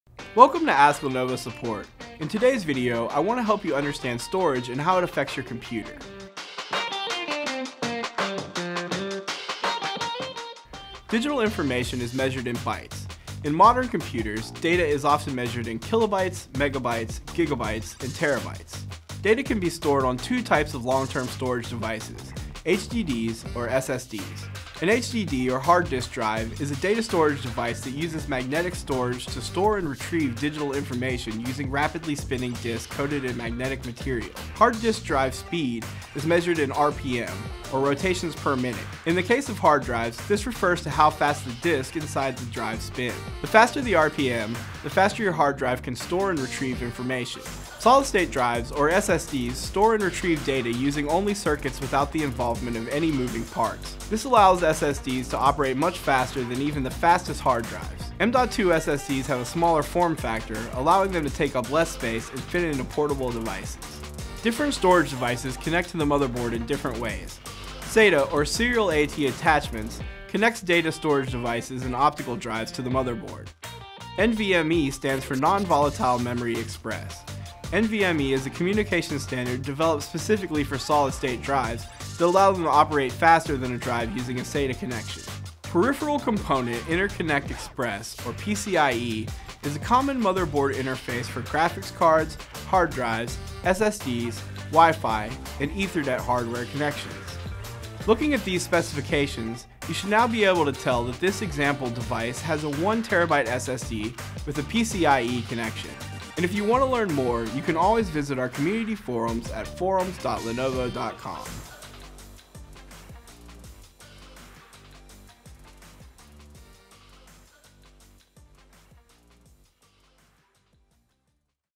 Ask Lenovo Support - Understanding Computer Storage
In this video, we will help you understand the basics of computer storage and the differences between hard disk drives and solid state drives.